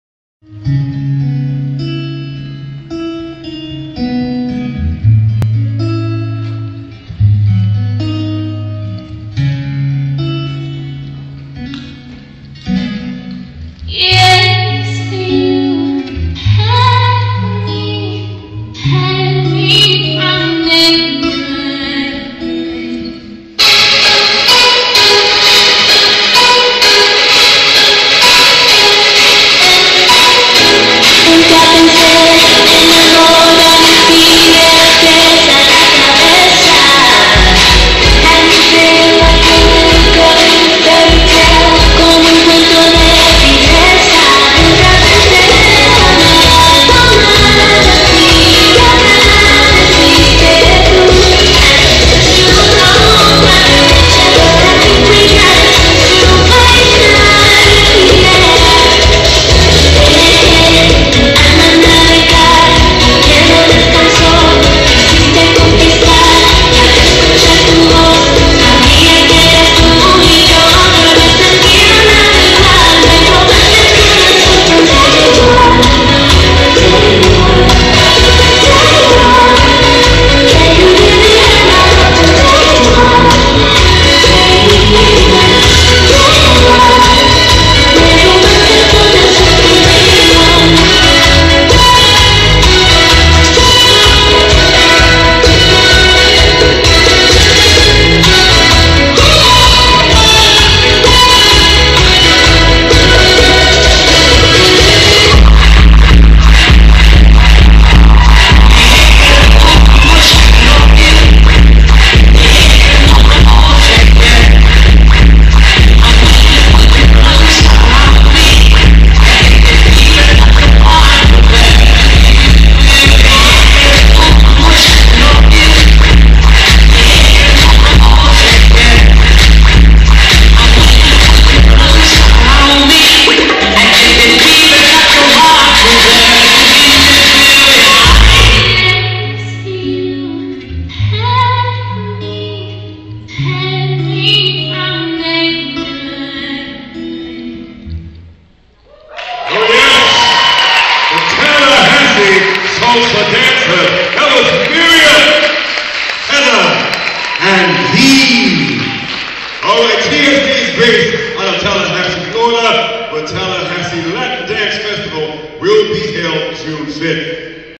Tallahassee Salsa Dancers Ladies Bachata team
TSD Ladies bachata team at 2015 Unity Dance Festival